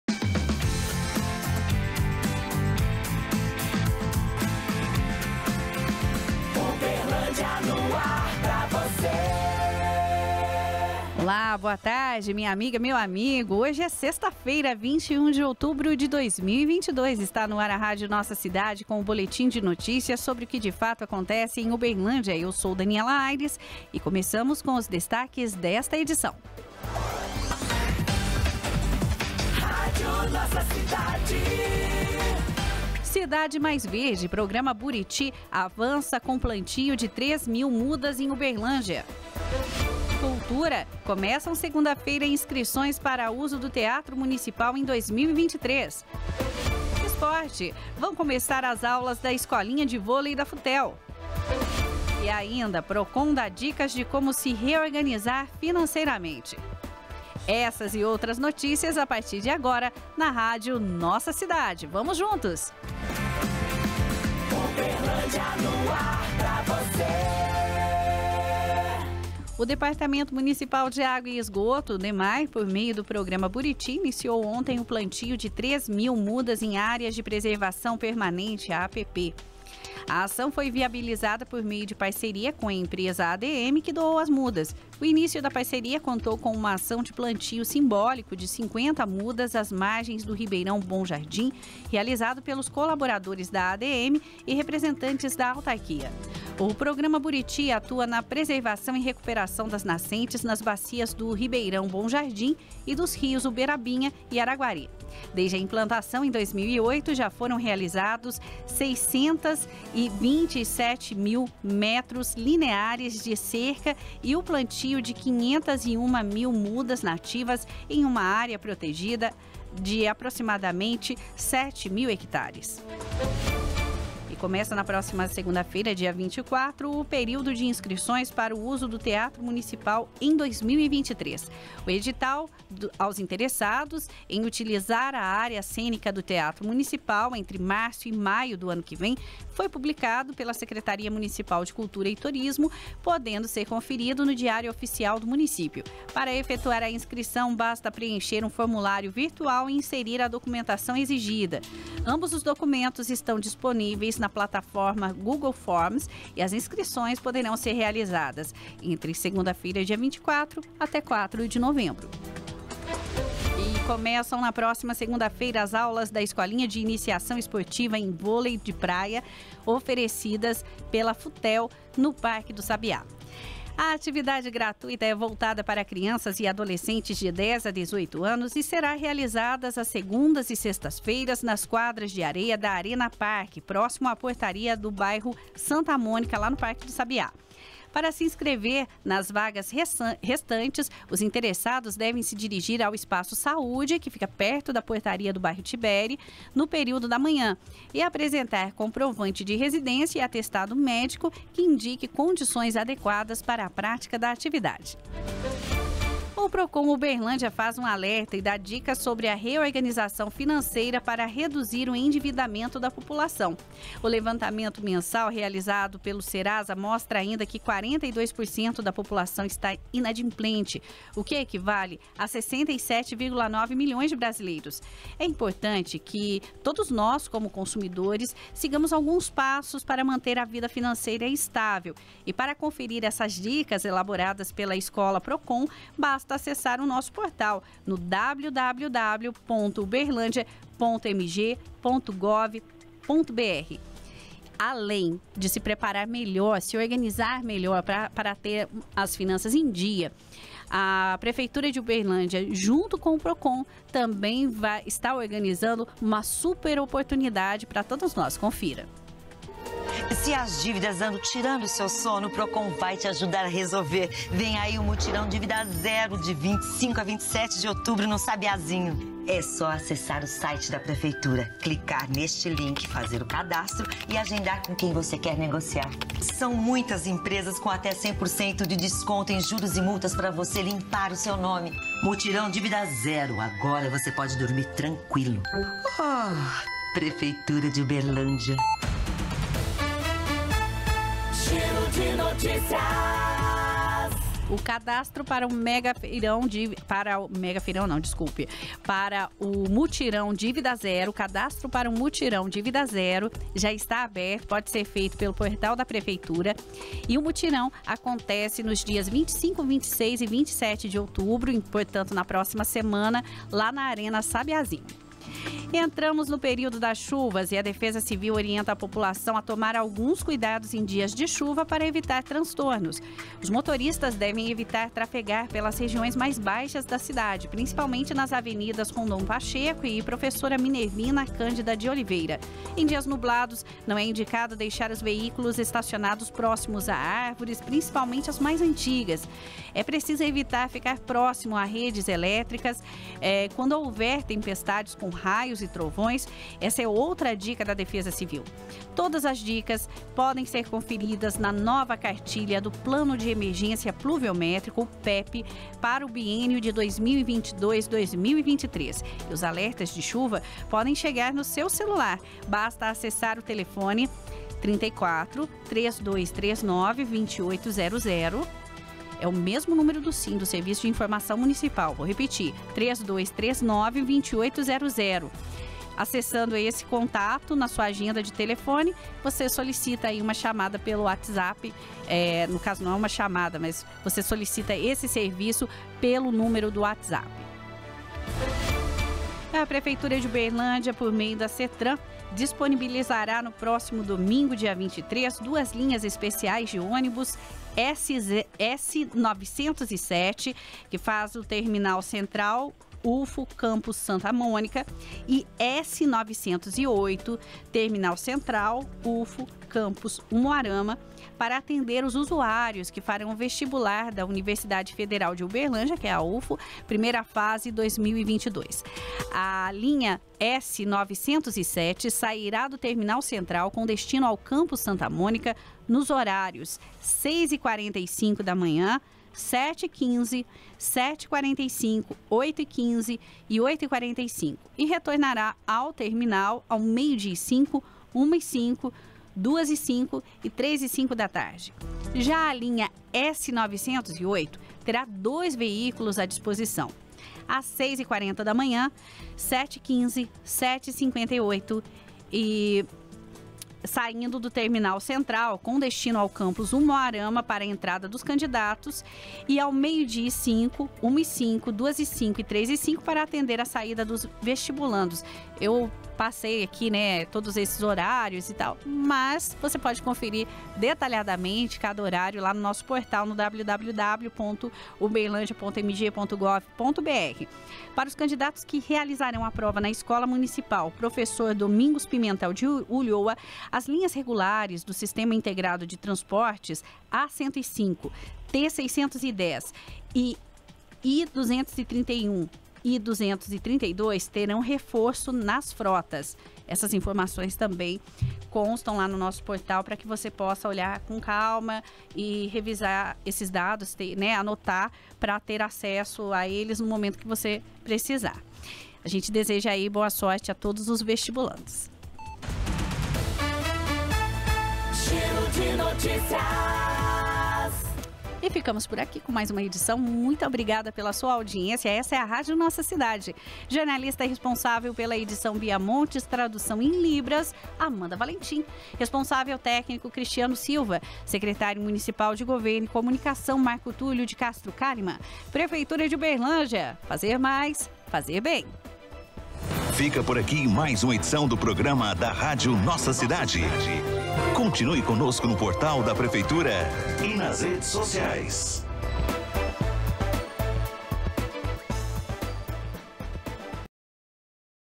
#816: Cidade mais verde programa Buriti avança com plantio de três mil mudas em Uberlândia
Começam segunda feira inscrições para uso do Teatro Municipal em 2023, Vão começar as aulas da escolinha de vôlei da Futel, Procon dá dicas de como se reorganizar financeiramente.